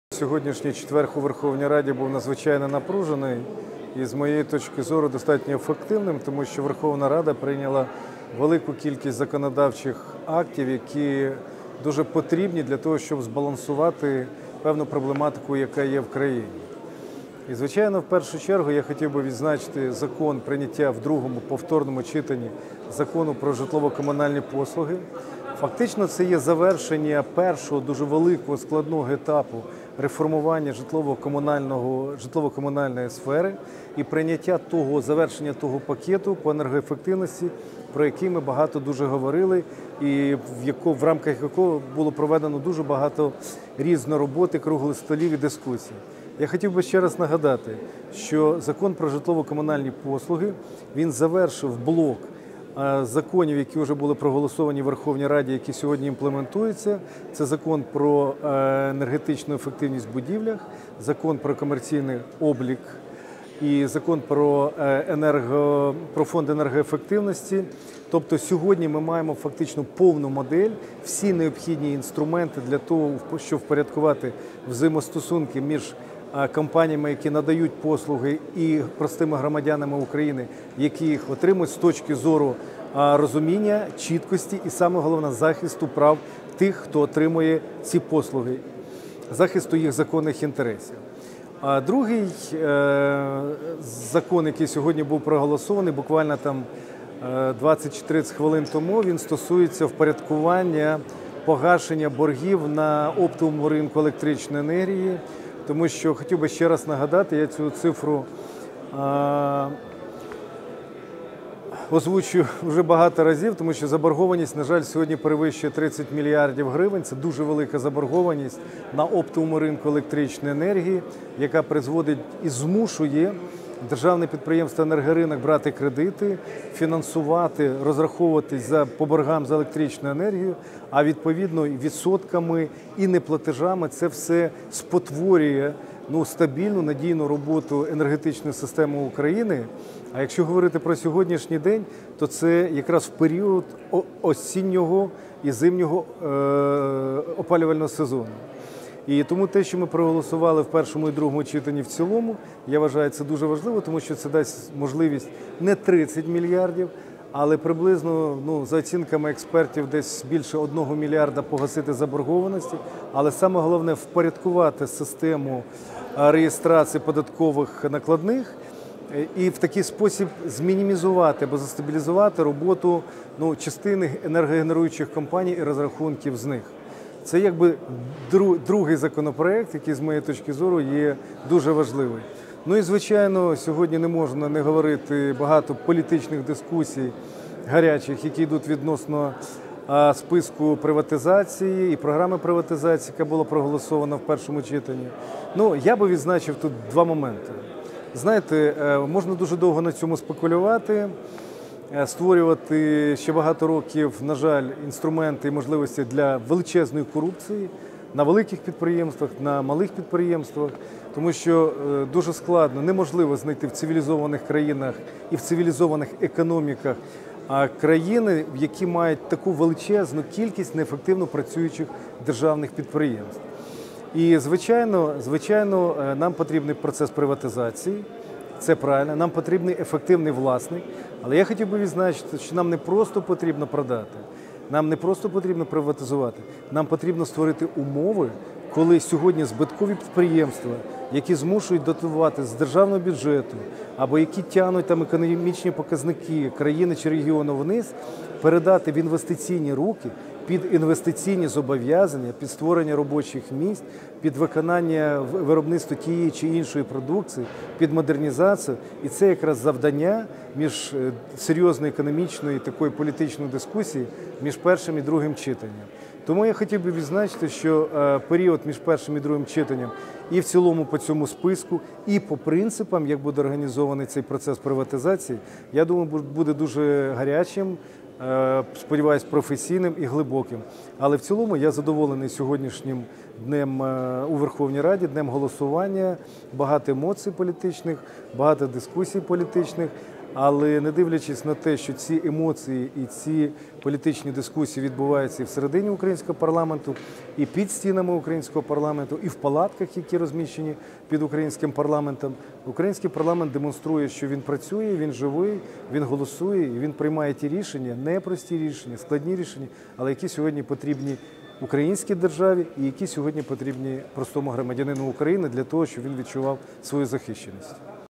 Пленарне засідання Верховної Ради України 9.11.2017
Сьогодні у Верховній Раді України був надзвичайно продуктивний день: про ухвалені законопроекти, які стосуються житлово-комунального господарства, електроенерегтичного сектору та питання приватизації державного майна